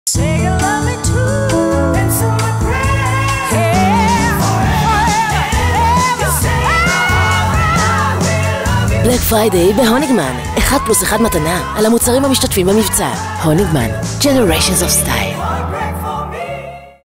Honigman - 1+1
חגיגת Black Friday בהוניגמן
1+1 מתנה על המוצרים המשתתפים במבצע